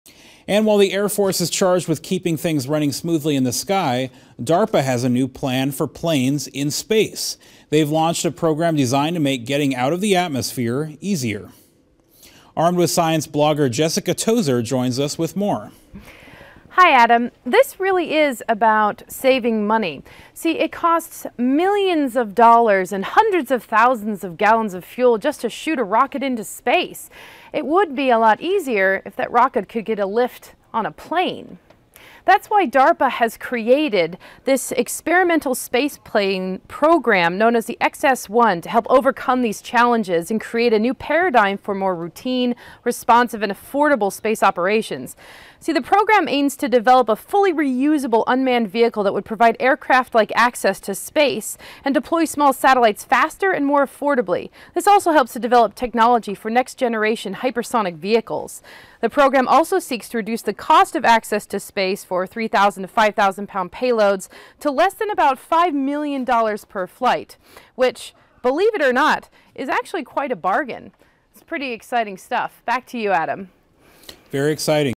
DARPA Introduces Space Plane Program XS-1
This DARPA video explains and depicts the Space Plane Program, and shows concept animations of what the XS-1 Spaceplane is expected to look like.